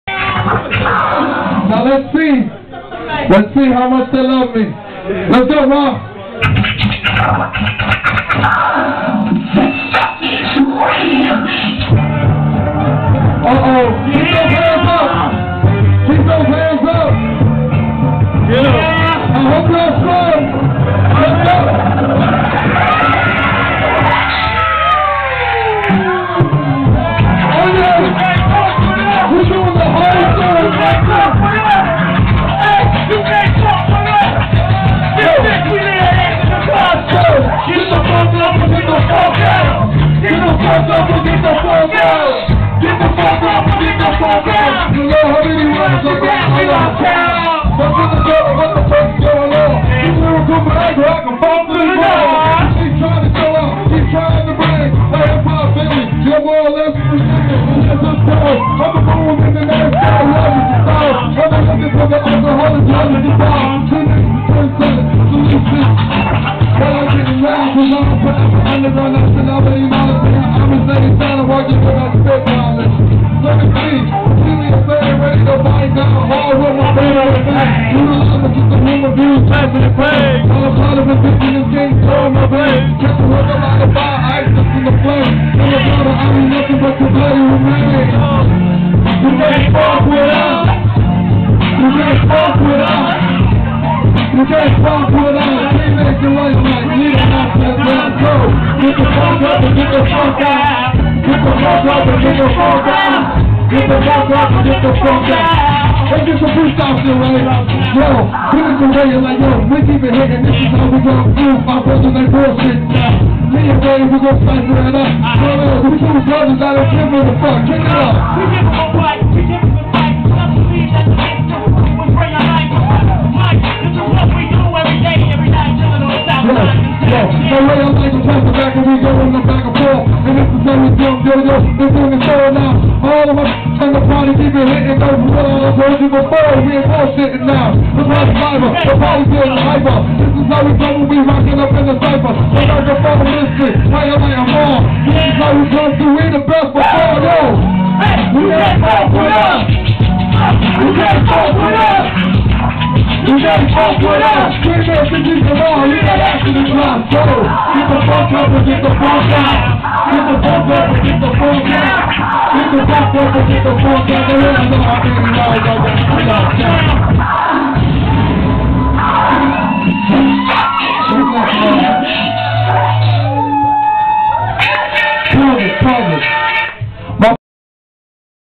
D-Nick The Microphone Misfit Crowd surfing
D-Nick & the crew were rocking an after set at the E. Palace and the felas got it cracking  D & the Ray of Light even started freestyling maybe cause D-Nick 4got the lyrics but this was a set the E. Palace wont 4get.